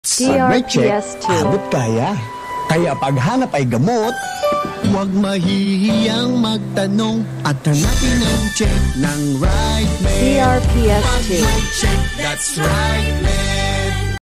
RiteMED Radio Ad 2024 15s with Coco Martin (Alternate Version)
Ito po ang 2024 Radio Ad ng RiteMED - Pag may check, that's the RITE ONE, that's RiteMED! (15-seconder alternate version)

DRPS Two 7471: RiteMED Radio Ad 2024 15s with Coco Martin (Alternate Version)